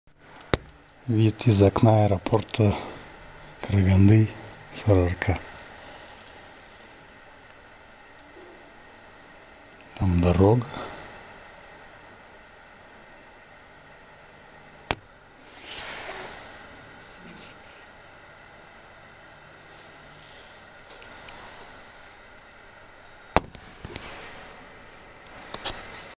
Вид из окна аэропорта Караганды Сары Арка на рассвете
Аэропорт города Караганды называется Сары Арка. Вид из огромного окна аэропорта - небо начинает окрашиваться в цвета, потому что светает. Наступает утро в аэропорту Караганды.